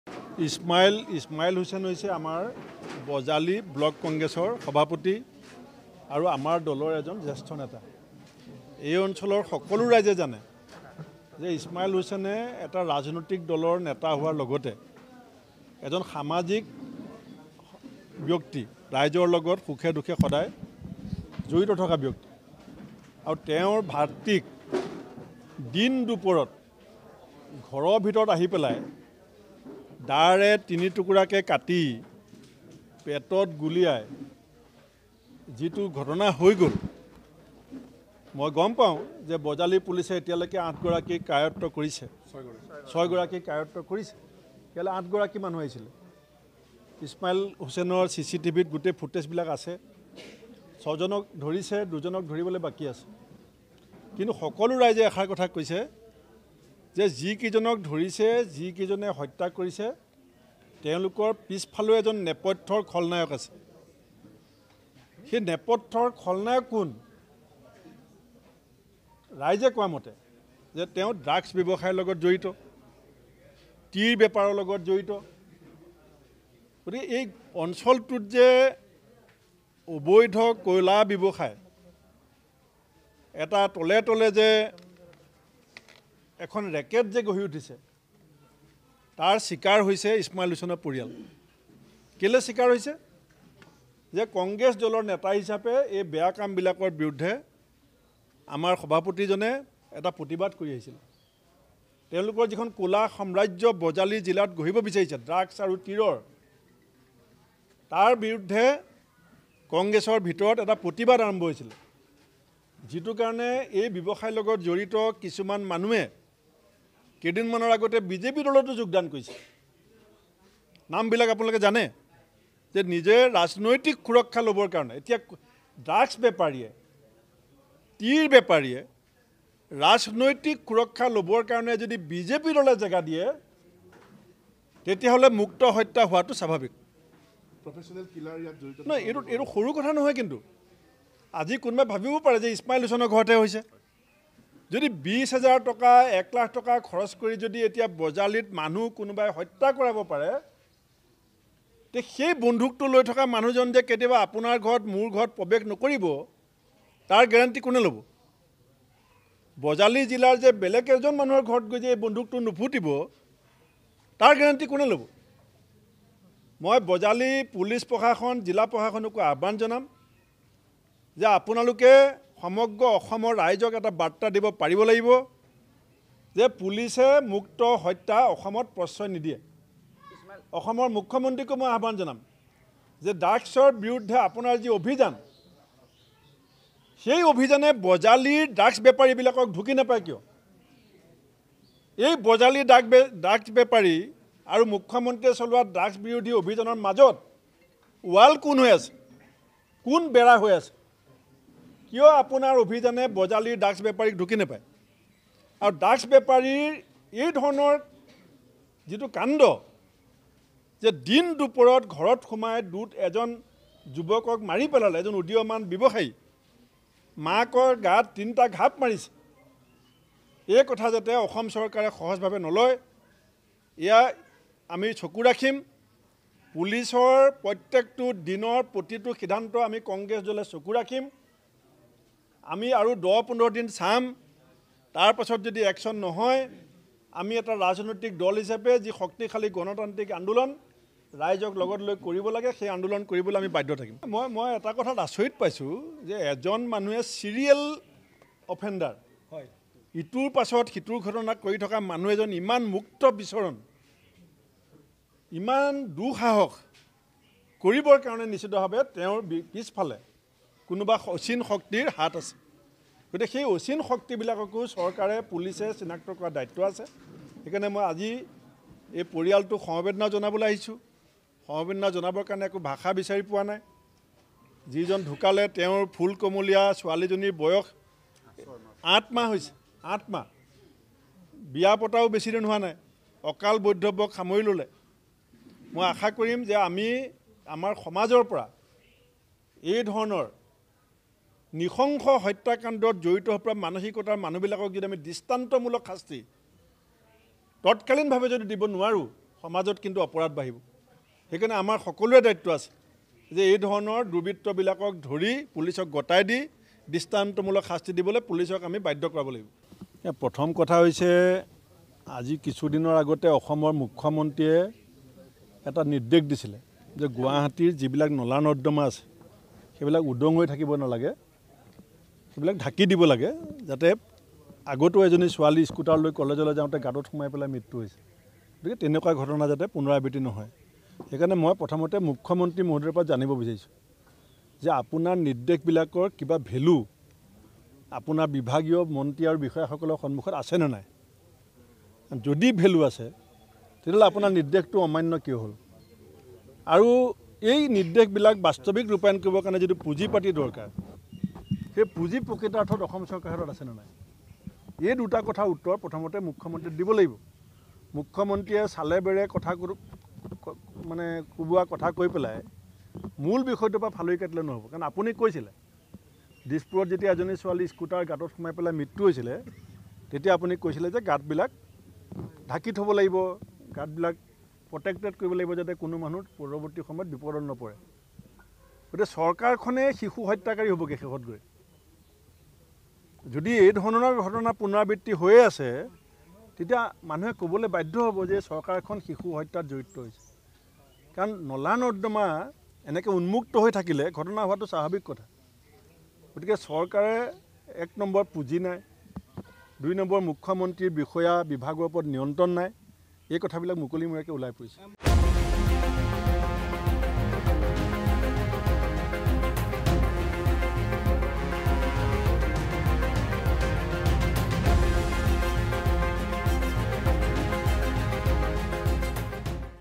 বজালীৰ ধুমাৰকুৰৰ মৃত ইব্ৰাহীম আলিৰ ঘৰত কংগ্ৰেছ সভাপতি ভূপেন বৰা
▶️ বজালীৰ ধুমাৰকুৰৰ মৃত ইব্ৰাহীম আলিৰ ঘৰত কংগ্ৰেছ সভাপতি ভূপেন বৰা। অপৰাধীক দৃষ্টান্তমূলক শাস্তি দিয়াৰ দাবী ভূপেন বৰাৰ।